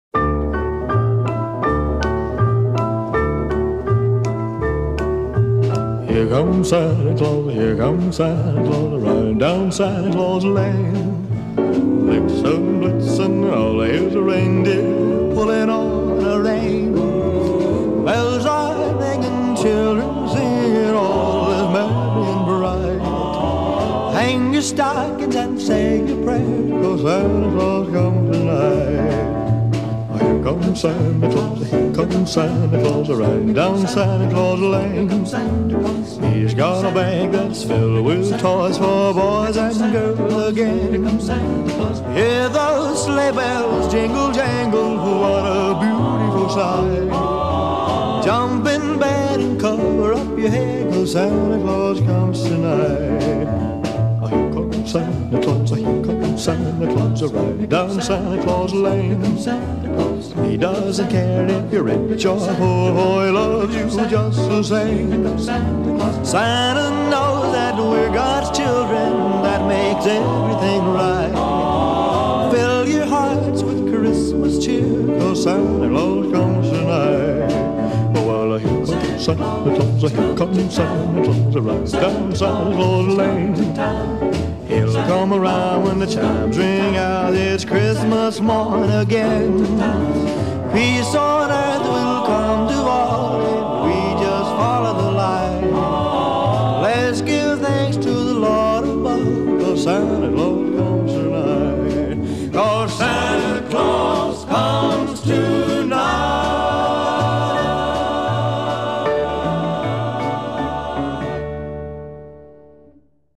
Santa Cool Bird
Cool Bird and employees of Les Schwab paid a visit to the Forgotten Children's Fund warehouse to help wrap toys T-Birds fans donated to the Fund at the first three home games in December.